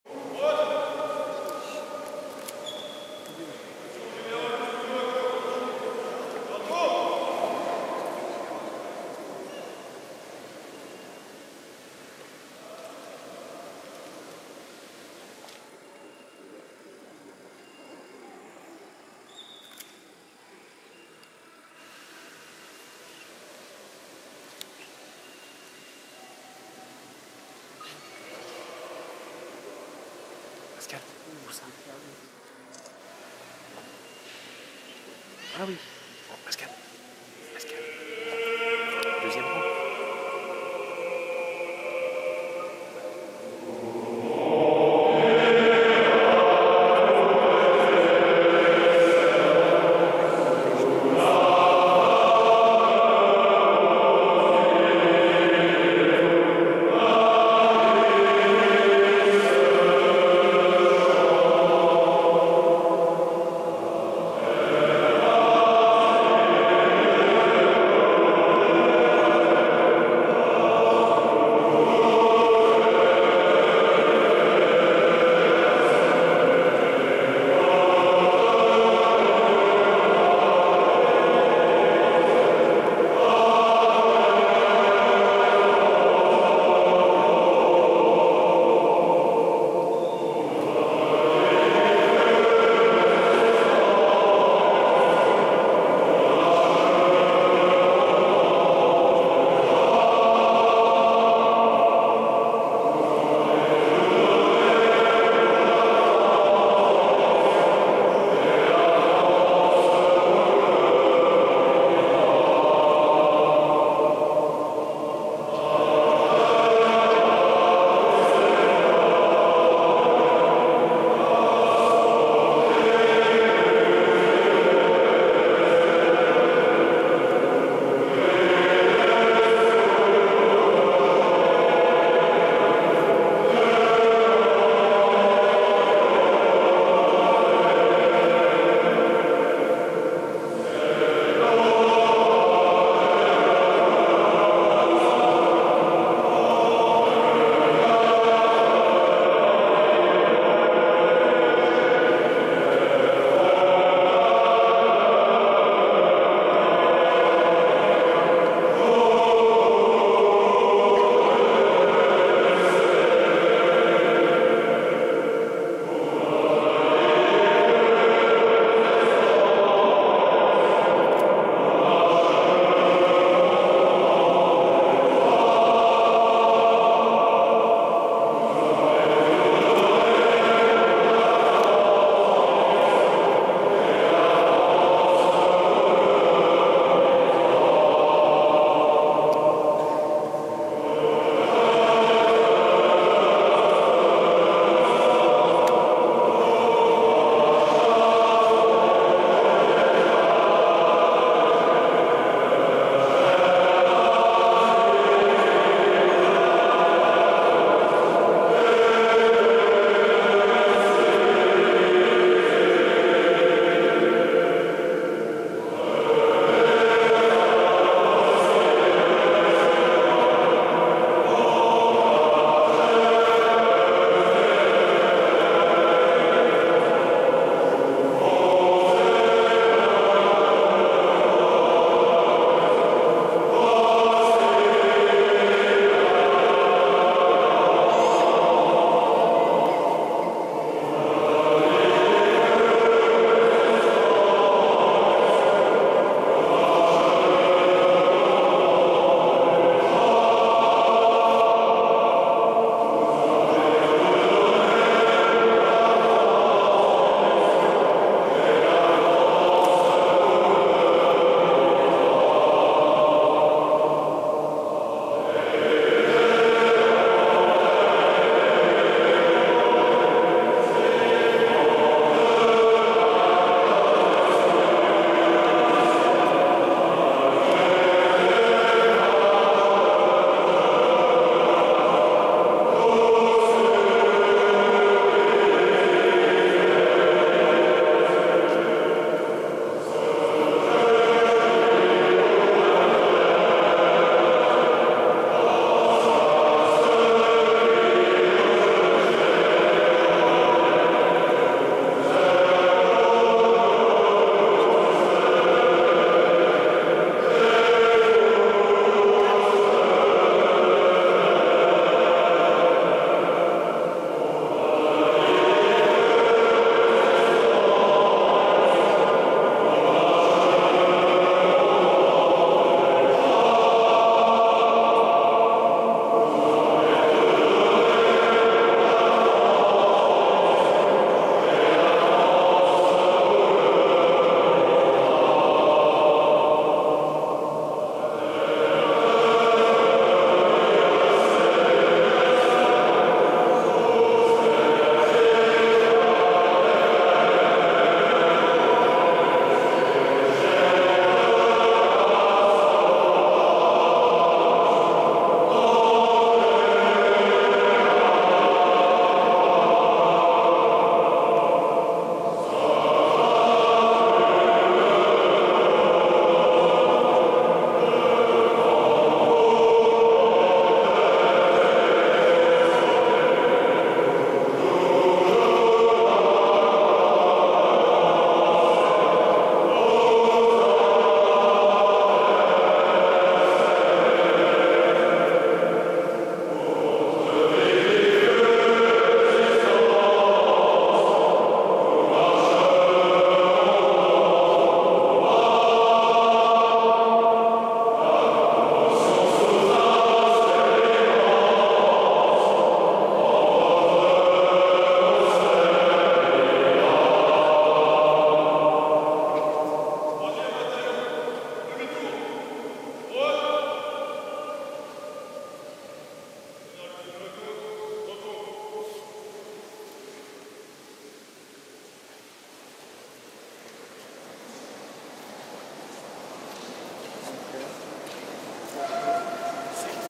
20171007 Chant Promotion Général Loustaunau Lacau tombeau Napoléon
Promotion Général Loustaunau-Lacau aux Invalides (tombeau de Napoléon) le 7 octobre 2018